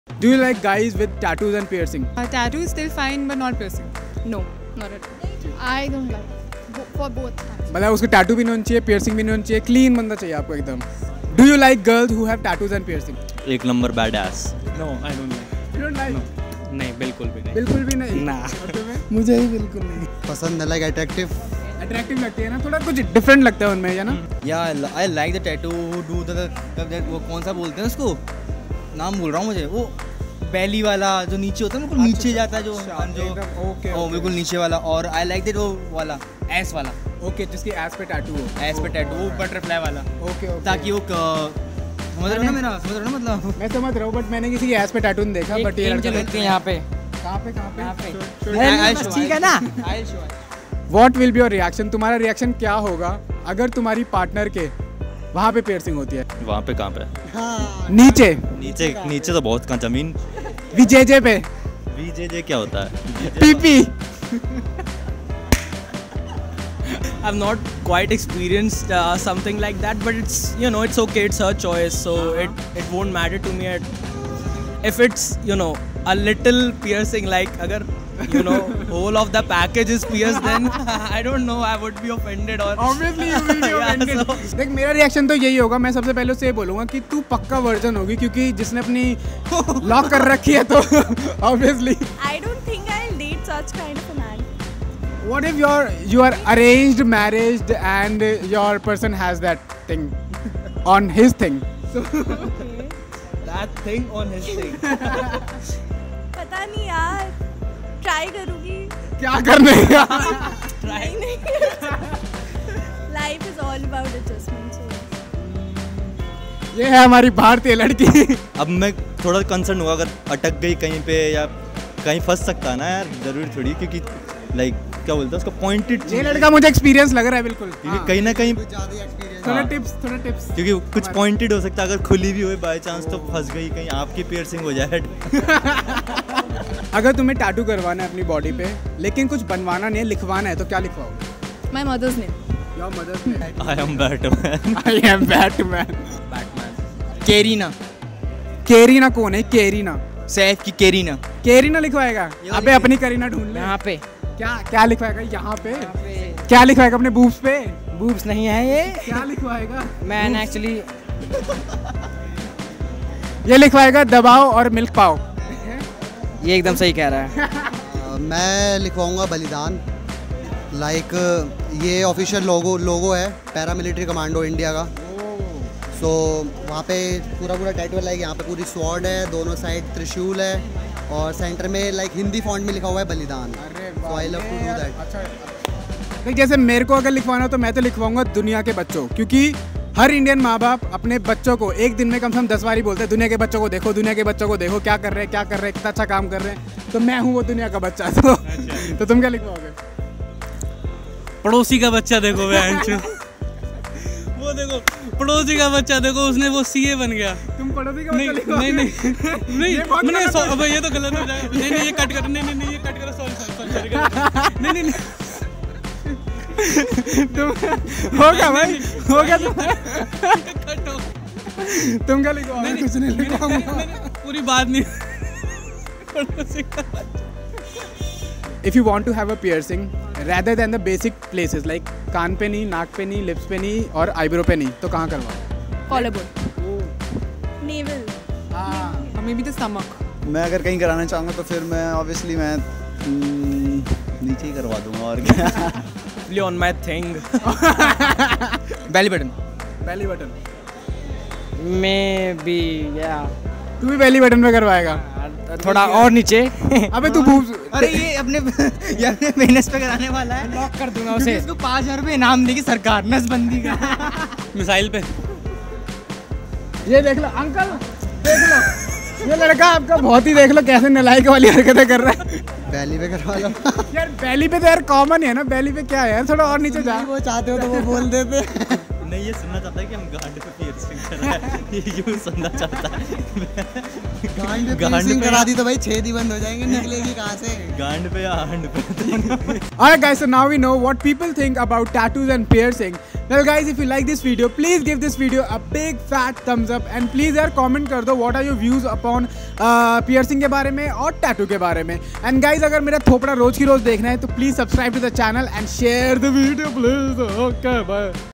DELHI ON PIERCING AND TATTOOS ON PRIVATE BODY PARTS | Crazy Duksh | Street Interview
Delhi people gave their opinion on tattoos and piercings. Hope you'll enjoy the video.